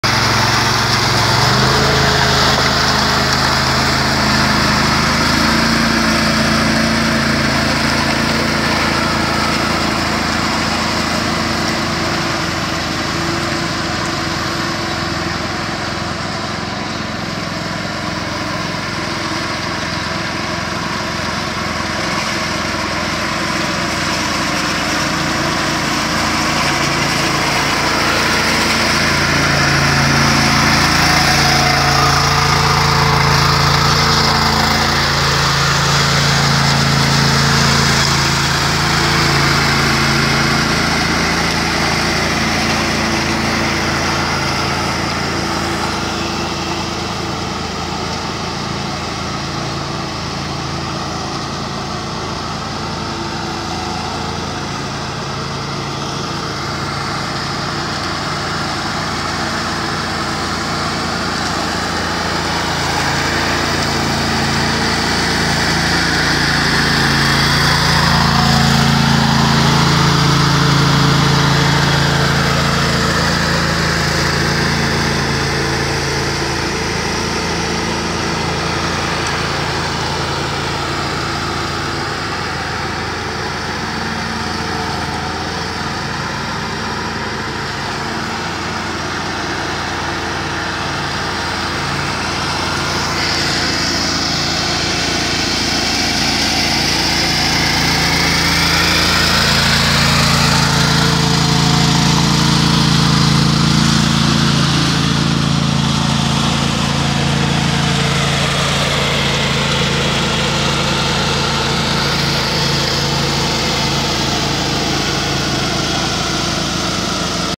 Pavimento industriale overlapper 90 no BARIKELL :/
Fase di lisciatura di un pavimento industriale con macchina lisciatrice doppia OL 90 ANTONELLO